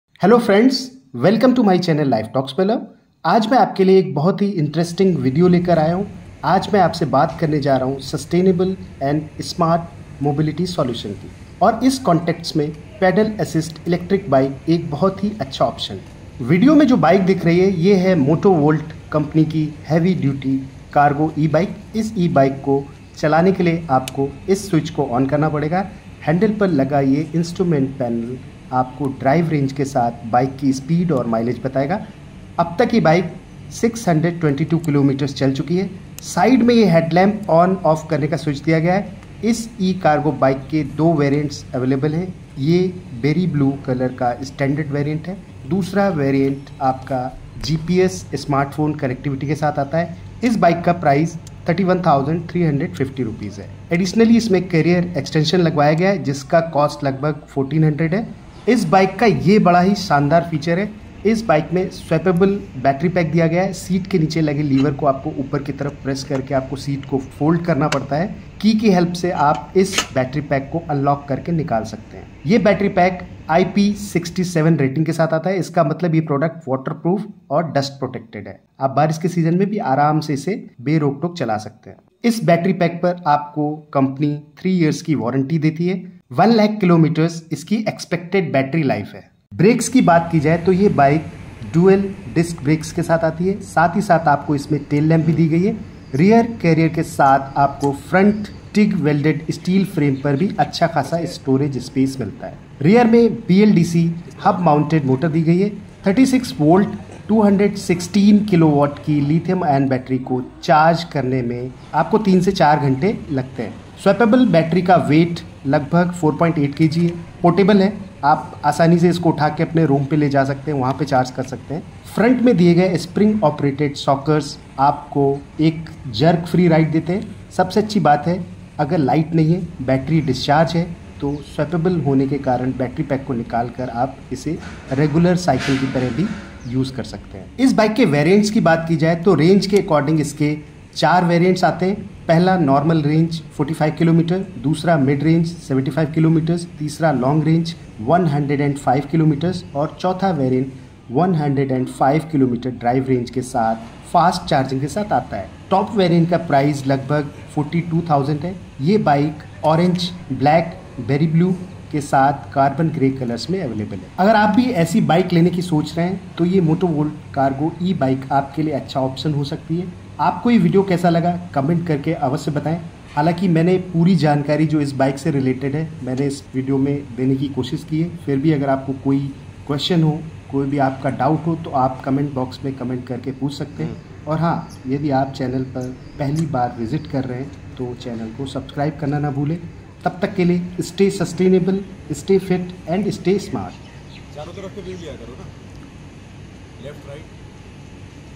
Power-Packed & Sustainable Ride: Motovolt Heavy-Duty Cargo E-Bike Review
Motovolt Heavy-Duty Cargo E-Bike